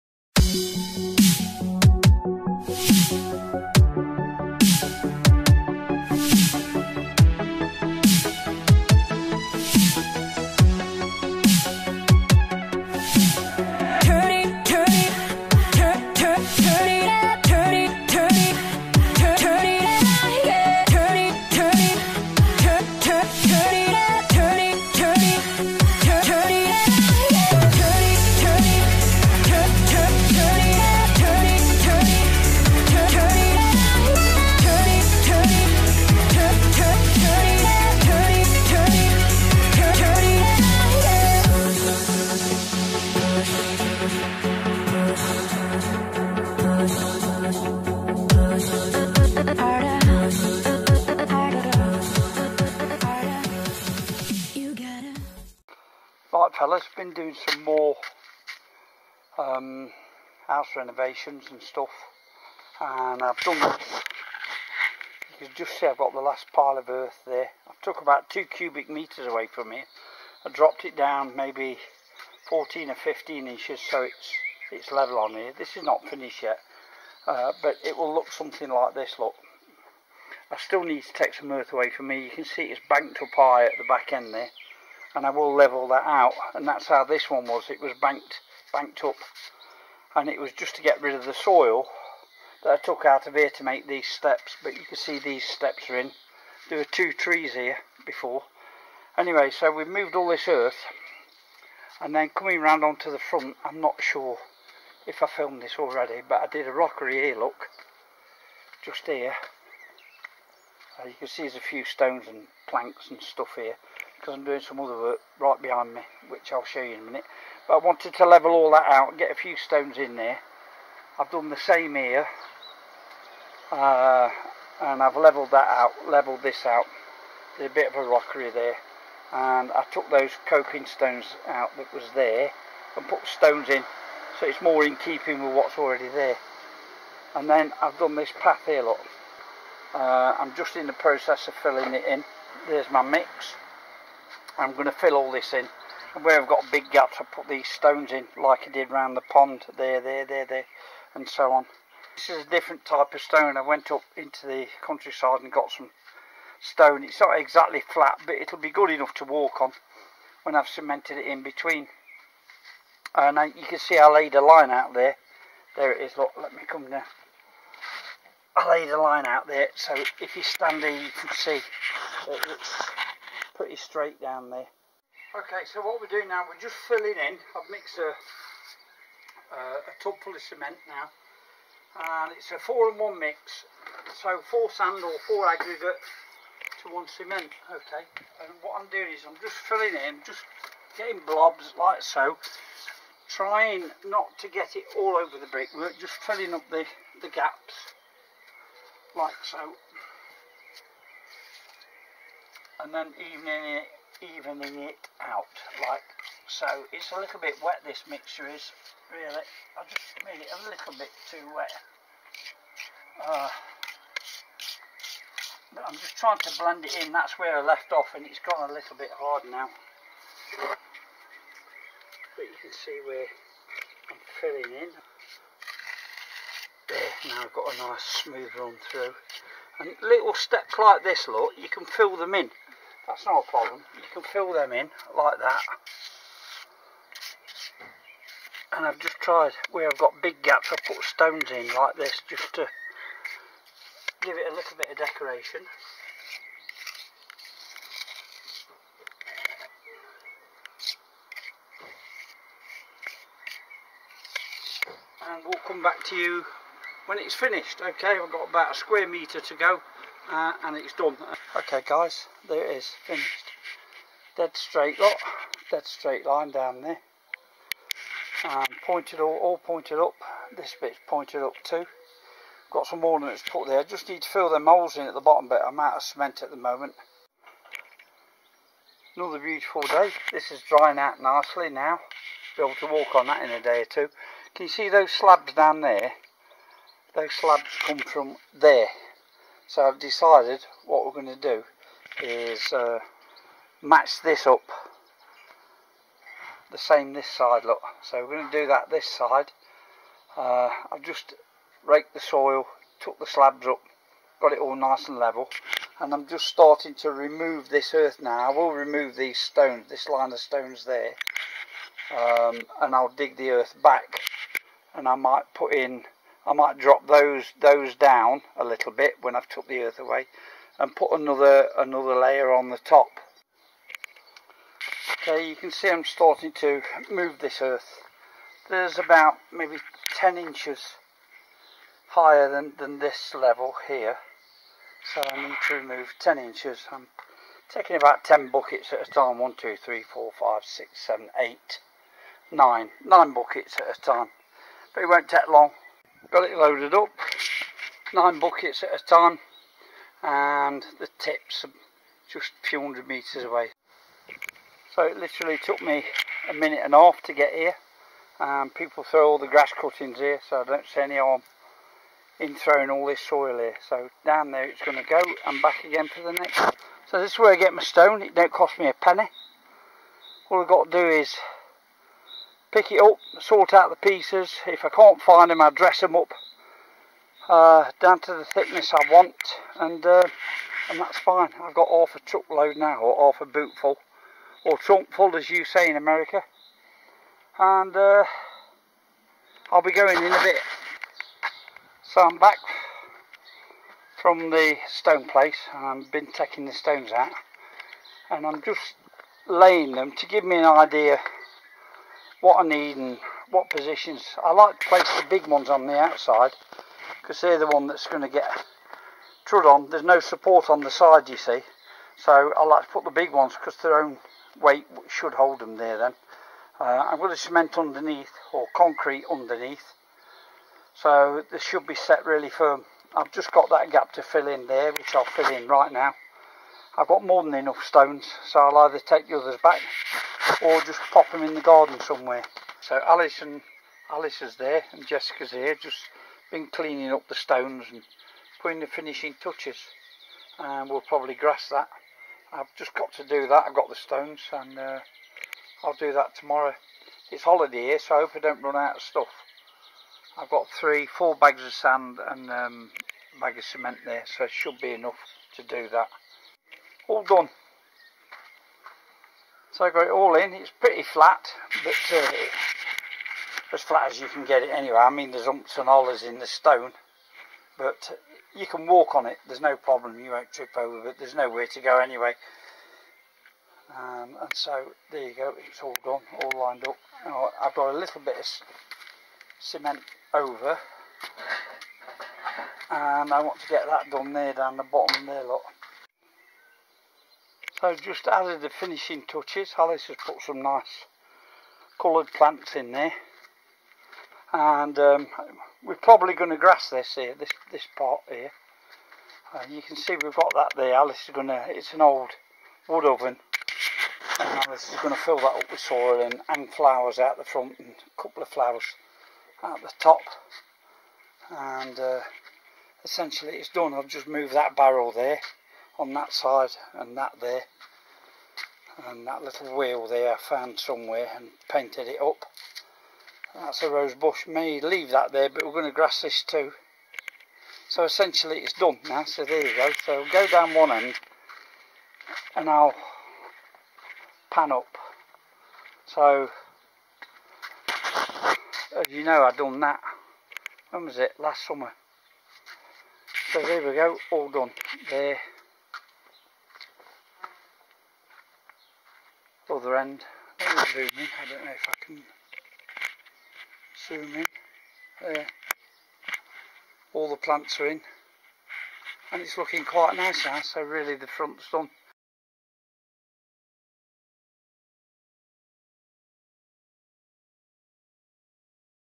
How to make a beautiful garden rockery and crazy paving pathway
A few tips on how to make a beautiful garden rockery and how to make crazy paving at no cost